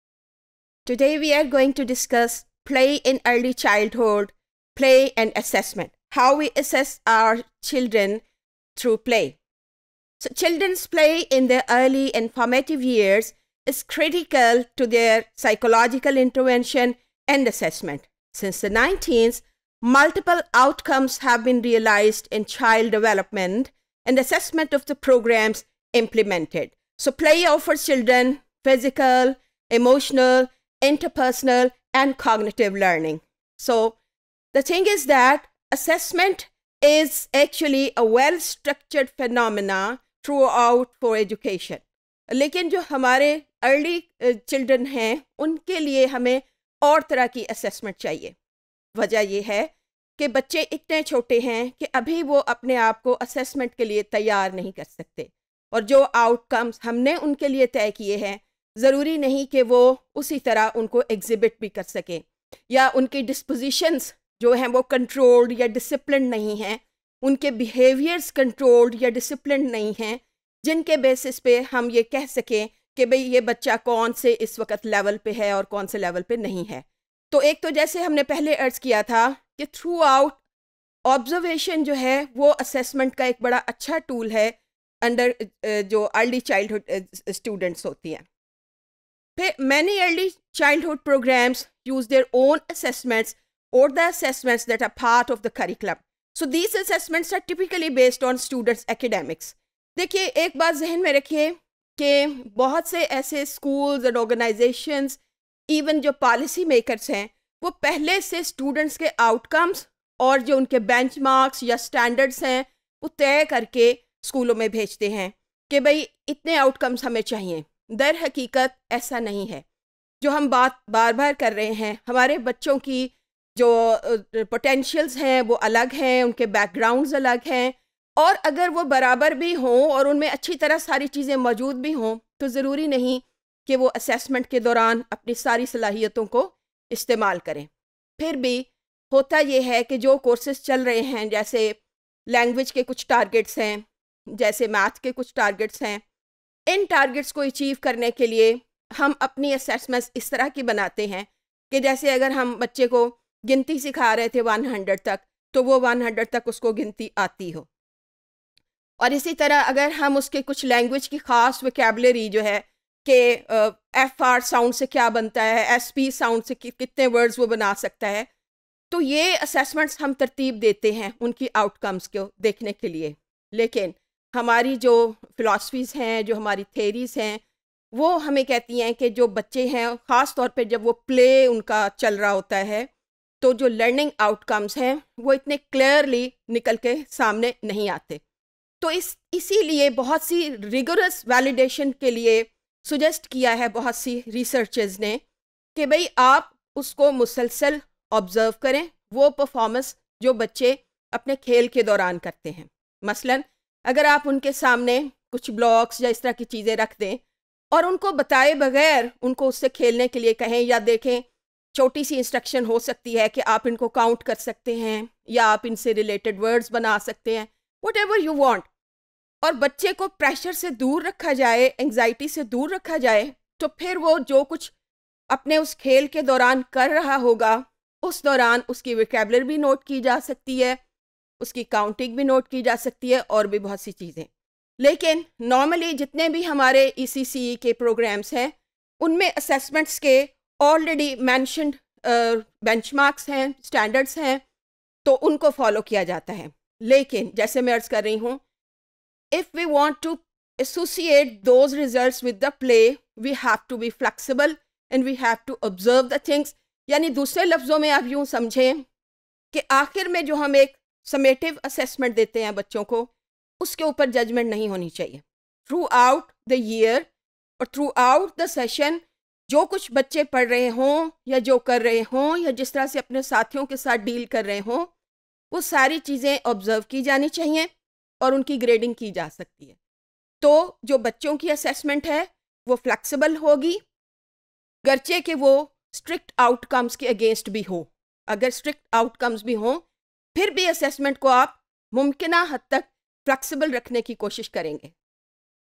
Play & Assessment | Trends and Issues in Early Childhood Education | ECE402_Topic060
Topic060 - Play in Early Childhood: Play & Assessment
by Dr. Afifa Khanam